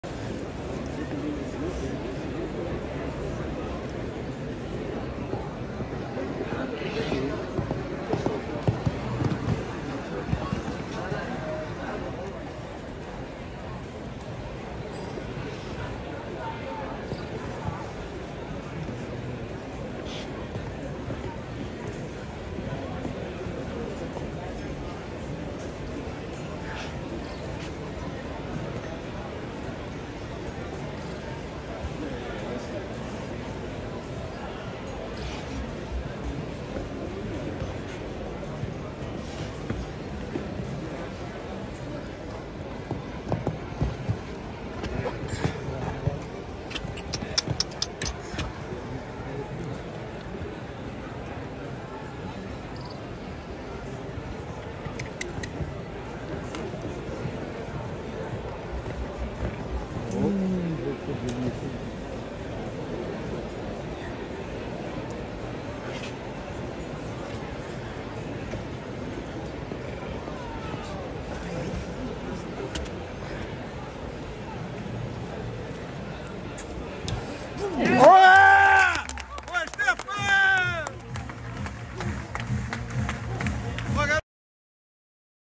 Stephan Barcha - Chevaux Primavera Império Egípcio (21/06/2024)
LONGINES League of Nations - Longines League of Nations™ Rotterdam
Int. team jumping competition over two rounds with jump-off (1.60 m)
Table A, FEI Art. 264+Annex VIII
Counts for the Longines Ranking - Group A - 1st round
Resultado - 0